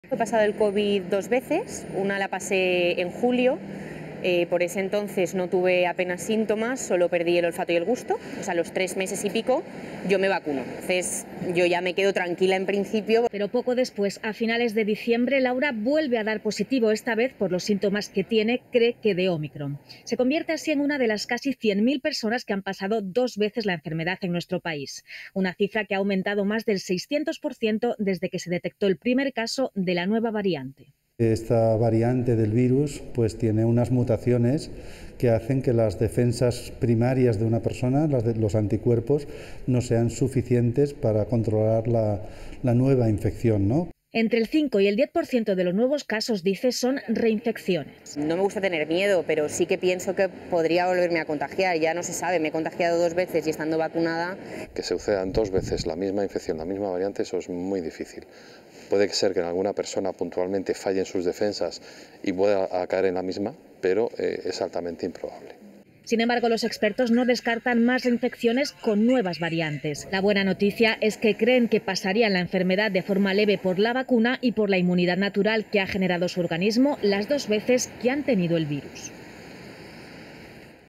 Las REINFECCIONES por COVID se SEXTUPLICAN en España por ÓMICRON I RTVE Noticias
Las reinfecciones por COVID-19 se han multiplicado por seis en España, y la causa hay que buscarla en ómicron, la variante dominante en estos momentos. Con la anterior, delta, muy pocos enfermos volvieron a infectarse.

El 24 de noviembre, cinco días antes de que se detectara el primer caso de ómicron en nuestro país, se habían reinfectado apenas 12.000 personas desde el principio de la pandemia. Ahora, en menos de dos meses, se han alcanzado casi las 100.000 reinfecciones. Eso sí, los expertos coinciden en que los síntomas son mucho más leves en la segunda infección. Gracias, en parte, a las vacunas.